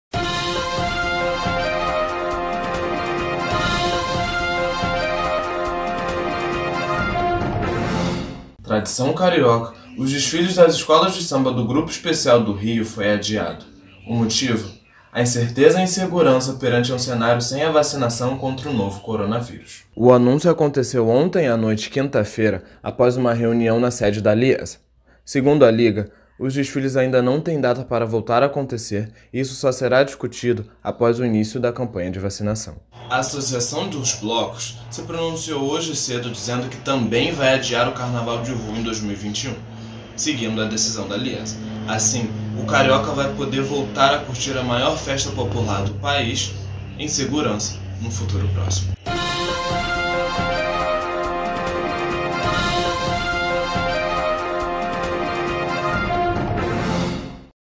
PLANTÃO - TV UVA NOTÍCIAS - CARNAVAL 2021 ADIADO
Não teremos Carnaval em fevereiro. A Liesa decidiu adiar o desfile das escolas de samba do Rio em 2021 em decorrência do novo Coronavirus, o evento ainda não tem data para acontecer.